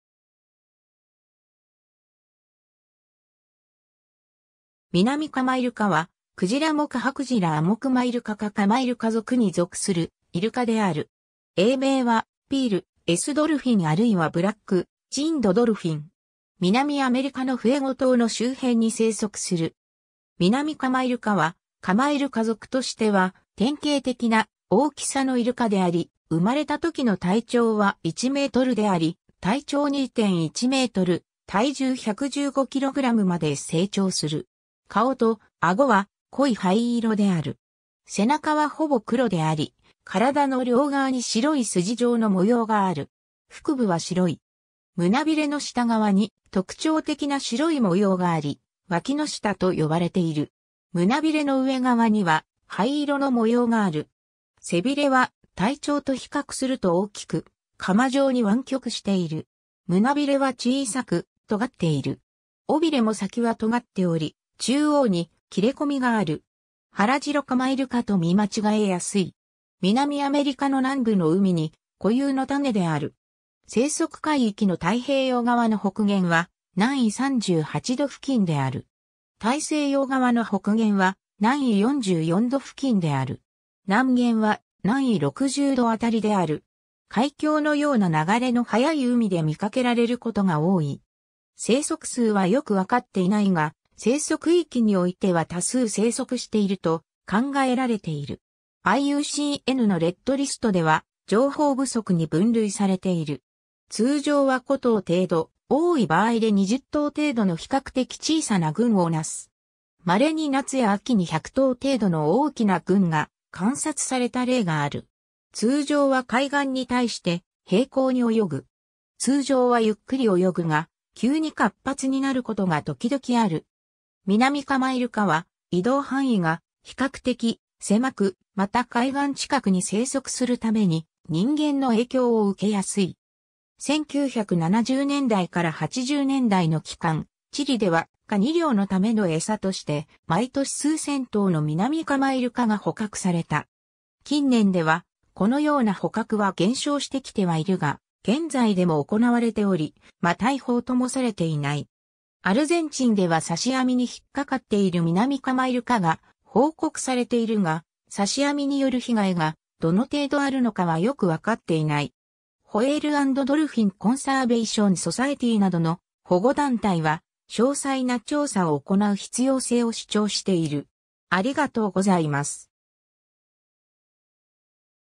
ミナミカマイルカ
ミナミカマイルカ（南鎌海豚、Lagenorhynchus australis）はクジラ目ハクジラ亜目マイルカ科カマイルカ属に属するイルカである。
英名はPeale's DolphinあるいはBlack-chinned Dolphin。
南アメリカのフエゴ島の周辺に生息する。
 ミナミカマイルカはカマイルカ属としては典型的な大きさのイルカであり、産まれた時の体長は1mであり、体長2.1m、体重115kgまで成長する。
顔とあごは濃い灰色である。
背中はほぼ黒であり、体の両側に白い筋状の模様がある。
腹部は白い。
胸びれの下側に特徴的な白い模様があり、「腋の下」と呼ばれている。
胸びれの上側には灰色の模様がある。
背びれは体長と比較すると大きく、鎌状に湾曲している。
胸びれは小さく、尖っている。
尾びれも先は尖っており、中央に切れ込みがある。
ハラジロカマイルカと見間違えやすい。
 南アメリカの南部の海に固有の種である。
生息海域の太平洋側の北限は南緯38度付近（バルディビア、チリあたり）である。
大西洋側の北限は南緯44度付近（Golfo San Jorge、アルゼンチンあたり）である。
南限は南緯60度あたりである。
海峡のような流れの速い海で見かけられることが多い。
 生息数は良くわかっていないが、生息域においては多数生息していると考えられている。
 IUCNのレッドリストでは「情報不足」 (DD - Data Deficient) に分類されている。
 通常は5頭程度、多い場合で20頭程度の比較的小さな群を成す。
稀に夏や秋に100頭程度の大きな群が観察された例がある。
通常は海岸に対して平行に泳ぐ。
通常はゆっくり泳ぐが、急に活発になることが時々ある。
 ミナミカマイルカは移動範囲が比較的狭く、また海岸近くに棲息するために、人間の影響を受けやすい。
1970年代から80年代の期間、チリではカニ漁のための餌として、毎年数千頭のミナミカマイルカが捕獲された。
近年ではこのような捕獲は減少してきてはいるが、現在でも行われており、また違法ともされていない。
 アルゼンチンでは刺し網に引っかかっているミナミカマイルカが報告されているが、刺し網による被害がどの程度あるのかは良くわかっていない。
Whale and Dolphin Conservation Societyなどの保護団体は、詳細な調査を行う必要性を主張している。